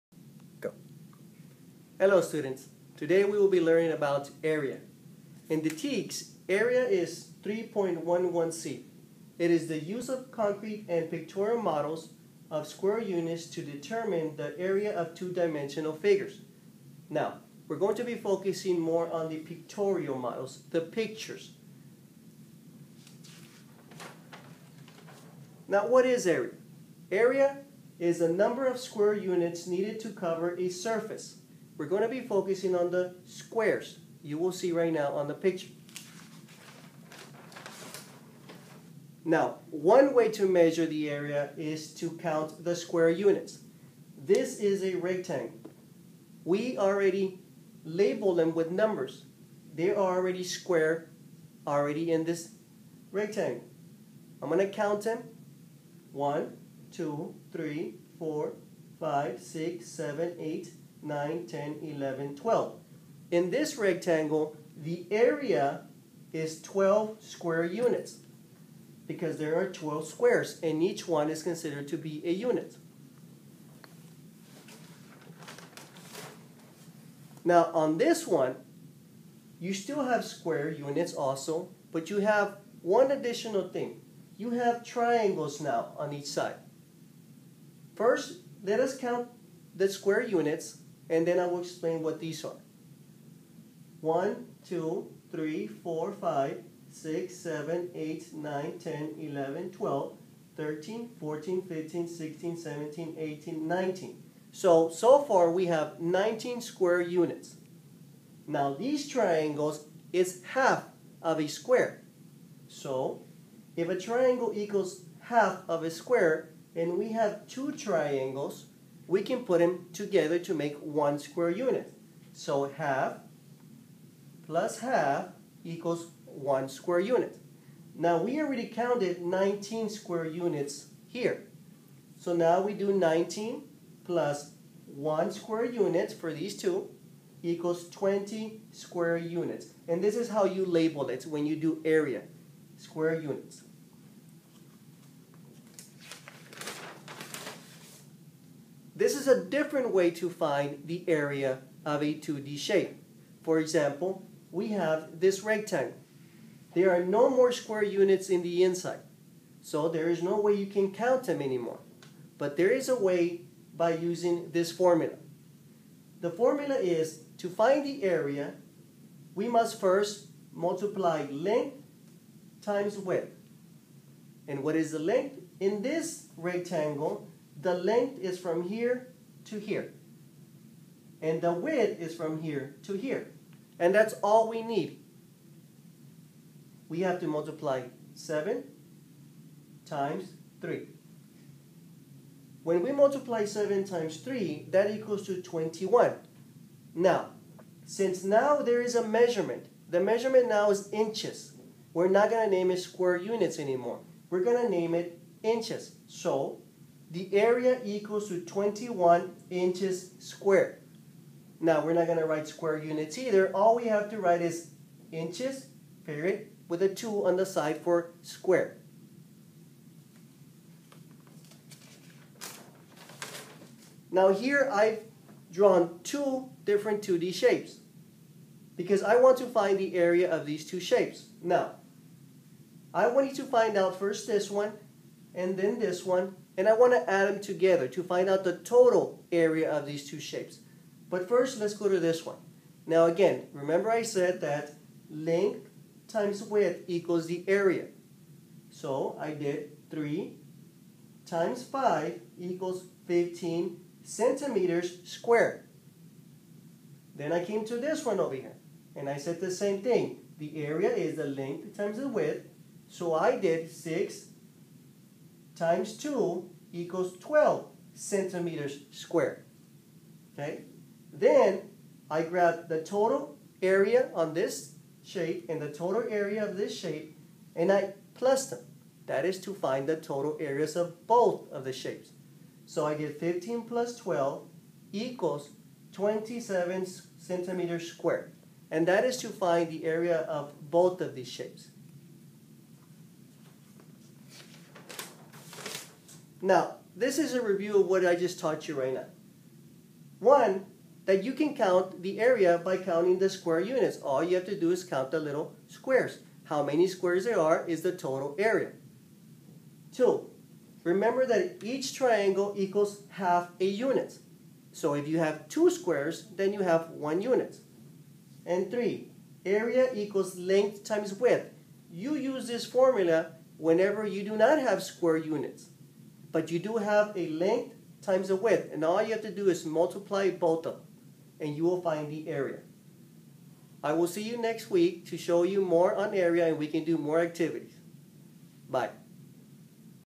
M Ybarra Third Grade Area
E. Quinones gives a lesson on Area.